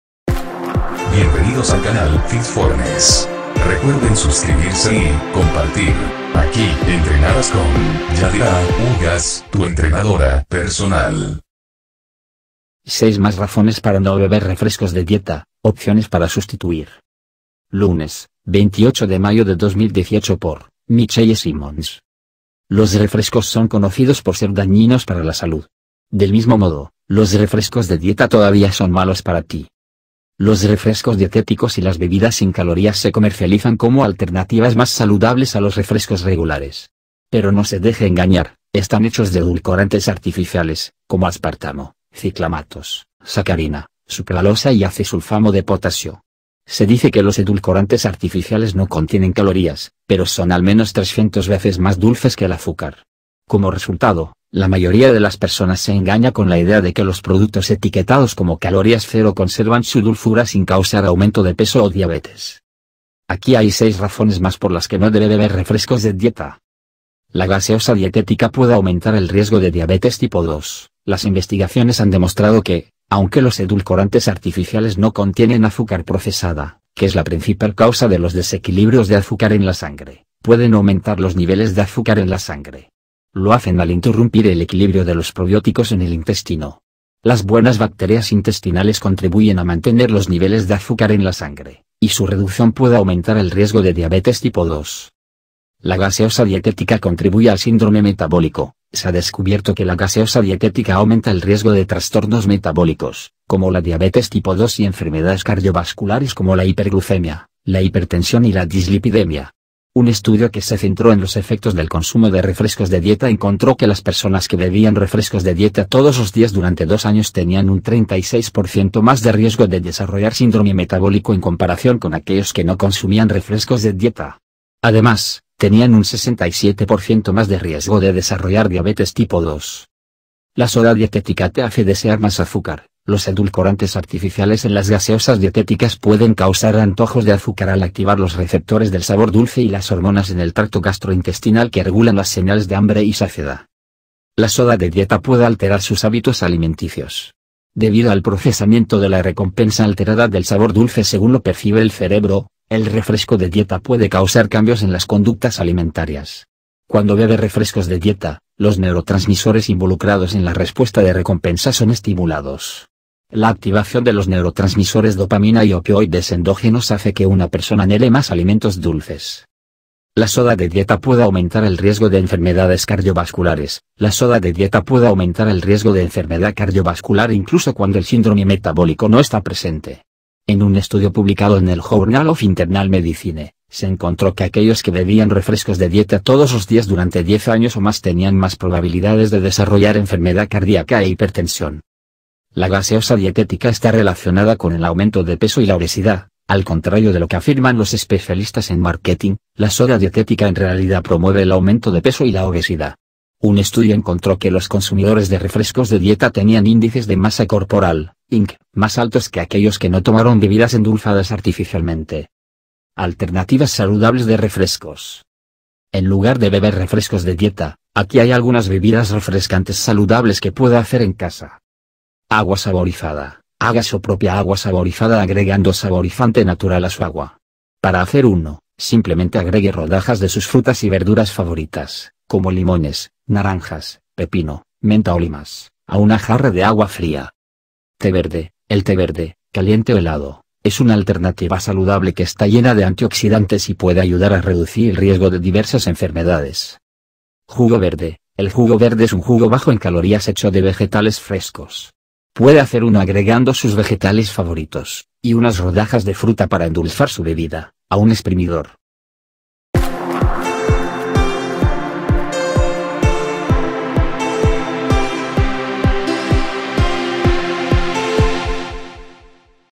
6 razones para NO beber refrescos de dieta, opciones para sustituir
Fit Fornes -
Yadira Ugas

6 razones para NO beber refrescos de dieta, opciones para sustituir.
Lunes, 28 de mayo de 2018 por: Michelle Simmons

Los refrescos son conocidos por ser dañinos para la salud. Del mismo modo, los refrescos de dieta todavía son malos para ti.

Los refrescos "dietéticos" y las bebidas sin calorías se comercializan como alternativas "más saludables" a los refrescos regulares. Pero no se deje engañar, están hechos de edulcorantes artificiales, como aspartamo, ciclamatos, sacarina, sucralosa y acesulfamo de potasio. Se dice que los edulcorantes artificiales no contienen calorías, pero son al menos 300 veces más dulces que el azúcar. Como resultado, la mayoría de las personas se engaña con la idea de que los productos etiquetados como calorías cero conservan su dulzura sin causar aumento de peso o diabetes.

Aquí hay seis razones más por las que no debe beber refrescos de dieta:

La gaseosa dietética puede aumentar el riesgo de diabetes tipo 2: las investigaciones han demostrado que, aunque los edulcorantes artificiales no contienen azúcar procesada, que es la principal causa de los desequilibrios de azúcar en la sangre, pueden aumentar los niveles de azúcar en la sangre. Lo hacen al interrumpir el equilibrio de los probióticos en el intestino. Las buenas bacterias intestinales contribuyen a mantener los niveles de azúcar en la sangre, y su reducción puede aumentar el riesgo de diabetes tipo 2.
La gaseosa dietética contribuye al síndrome metabólico: se ha descubierto que la gaseosa dietética aumenta el riesgo de trastornos metabólicos, como la diabetes tipo 2 y enfermedades cardiovasculares como la hiperglucemia, la hipertensión y la dislipidemia. Un estudio que se centró en los efectos del consumo de refrescos de dieta encontró que las personas que bebían refrescos de dieta todos los días durante dos años tenían un 36 por ciento más de riesgo de desarrollar síndrome metabólico en comparación con aquellos que no consumían refrescos de dieta. Además, tenían un 67 por ciento más de riesgo de desarrollar diabetes tipo 2.
La soda dietética te hace desear más azúcar: los edulcorantes artificiales en las gaseosas dietéticas pueden causar antojos de azúcar al activar los receptores del sabor dulce y las hormonas en el tracto gastrointestinal que regulan las señales de hambre y saciedad.
La soda de dieta puede alterar sus hábitos alimenticios. Debido al procesamiento de la recompensa alterada del sabor dulce según lo percibe el cerebro, el refresco de dieta puede causar cambios en las conductas alimentarias. Cuando bebe refrescos de dieta, los neurotransmisores involucrados en la respuesta de recompensa son estimulados. La activación de los neurotransmisores dopamina y opioides endógenos hace que una persona anhele más alimentos dulces.
La soda de dieta puede aumentar el riesgo de enfermedades cardiovasculares: la soda de dieta puede aumentar el riesgo de enfermedad cardiovascular incluso cuando el síndrome metabólico no está presente. En un estudio publicado en el Journal of Internal Medicine, se encontró que aquellos que bebían refrescos de dieta todos los días durante 10 años o más tenían más probabilidades de desarrollar enfermedad cardíaca e hipertensión.
La gaseosa dietética está relacionada con el aumento de peso y la obesidad: al contrario de lo que afirman los especialistas en marketing, la soda dietética en realidad promueve el aumento de peso y la obesidad. Un estudio encontró que los consumidores de refrescos de dieta tenían índices de masa corporal (IMC) más altos que aquellos que no tomaron bebidas endulzadas artificialmente.
Alternativas saludables de refrescos
En lugar de beber refrescos de dieta, aquí hay algunas bebidas refrescantes saludables que puede hacer en casa:

Agua saborizada: haga su propia agua saborizada agregando saborizante natural a su agua. Para hacer uno, simplemente agregue rodajas de sus frutas y verduras favoritas, como limones, naranjas, pepino, menta o limas, a una jarra de agua fría.
Té verde: el té verde, caliente o helado, es una alternativa saludable que está llena de antioxidantes y puede ayudar a reducir el riesgo de diversas enfermedades.
Jugo verde: el jugo verde es un jugo bajo en calorías hecho de vegetales frescos. Puede hacer uno agregando sus vegetales favoritos, y unas rodajas de fruta para endulzar su bebida, a un exprimidor.

No olviden ver nuestra lista de reproducción. Contamos con videos de